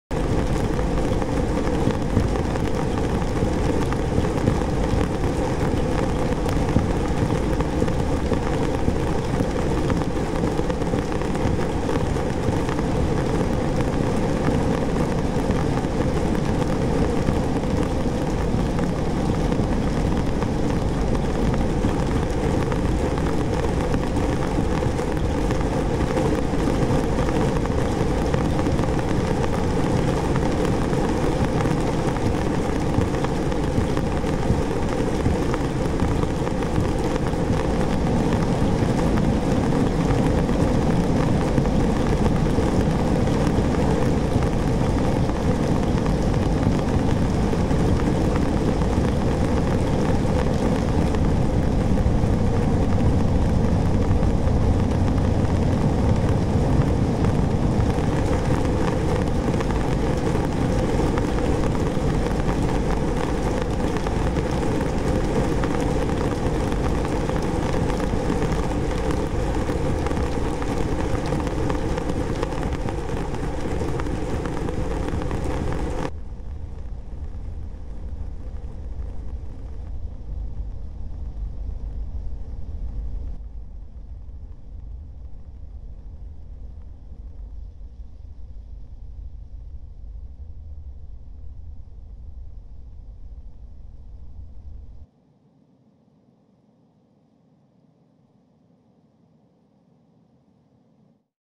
Zimoun Explores the Sense of Hearing With Immersive 329 Installation
Residing inside an industrial tank located in Dottikon, Switzerland, the 329 installation is composed of 329 motors with balls attached with string. As the balls endlessly move and hit the wall, a symphony of sounds are created for visitors to relish.

Video courtesy of Zimoun